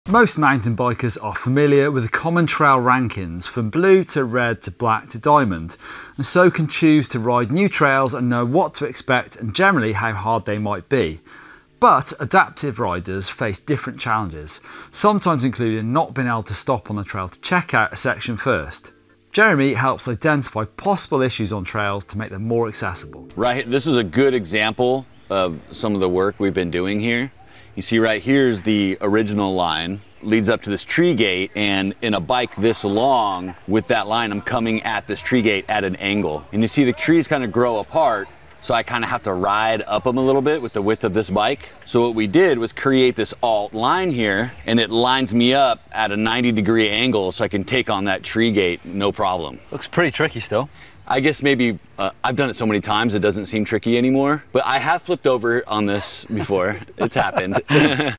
Adapting Trails For 3-Wheeled AMTBs!
Adapting trails for 3-wheeled AMTBs is brilliant for making trails accessible to everyone. Check out how small changes can make the biggest difference to a trail's accessibility!